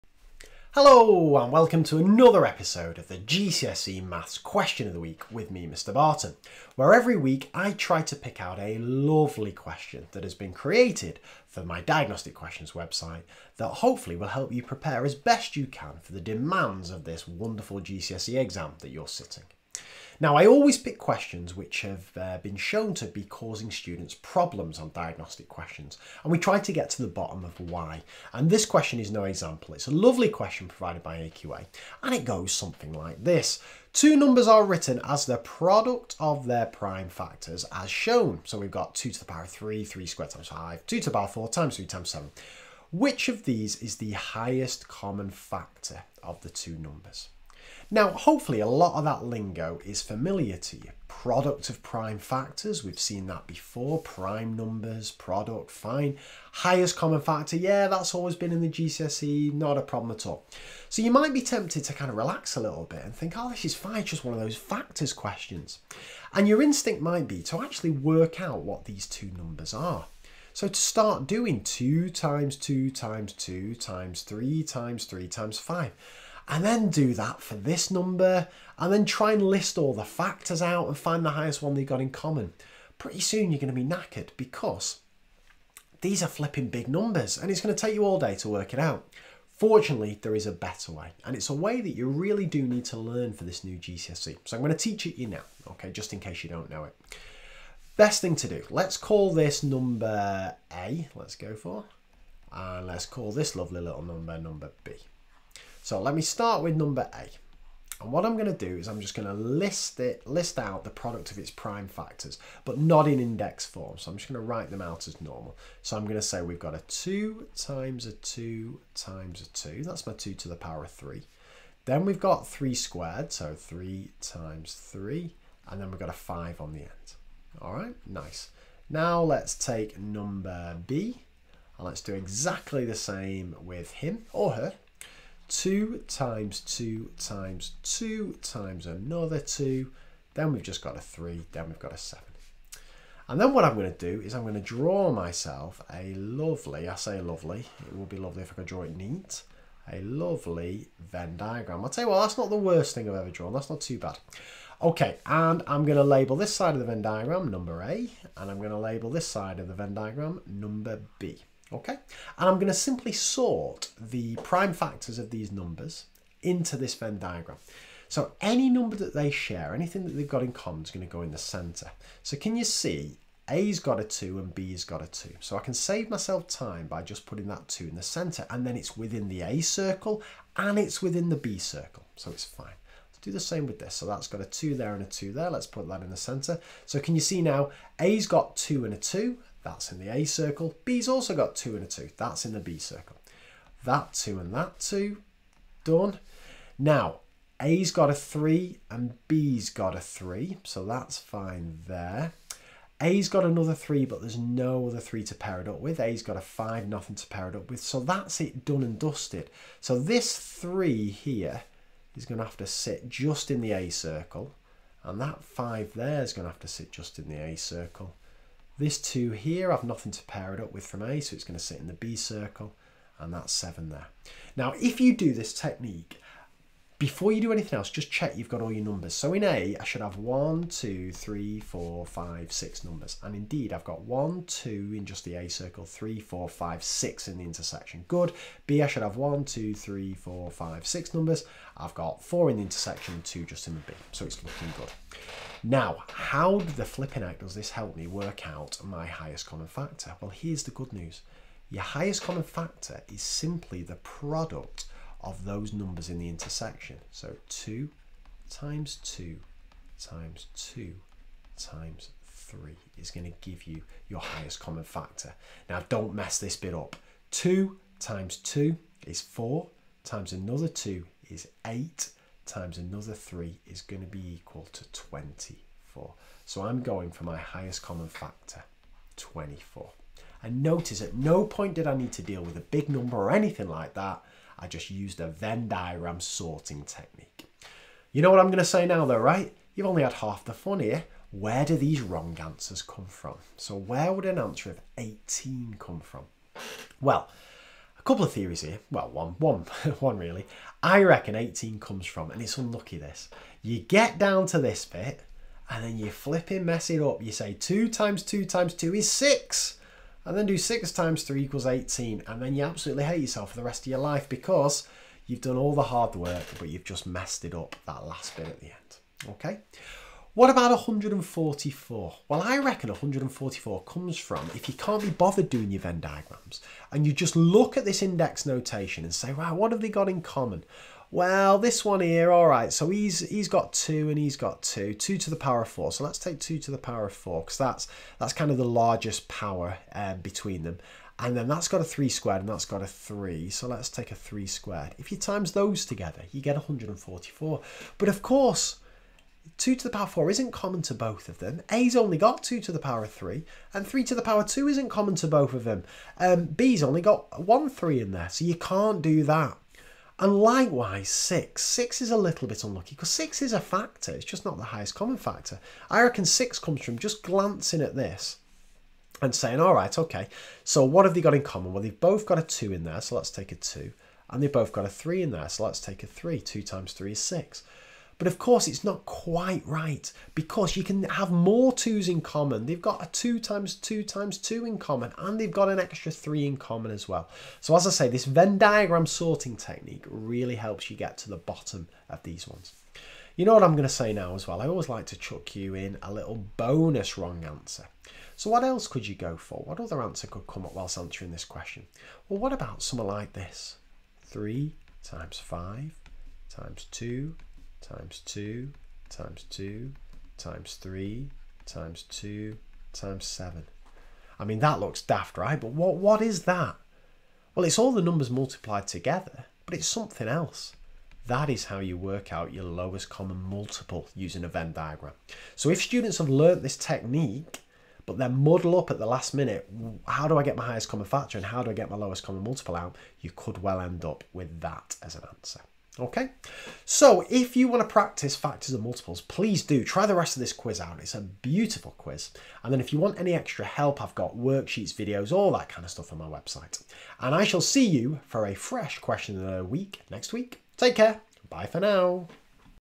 Highest Common Factor: GCSE Maths Question of the Week
on Mr Barton Maths for all the questions in this series, together with the worksheet to download and extra supporting resources.

This is a video for the new GCSE Maths specification on Highest Common Factor created using questions written by the awarding bodies for Diagnostic Questions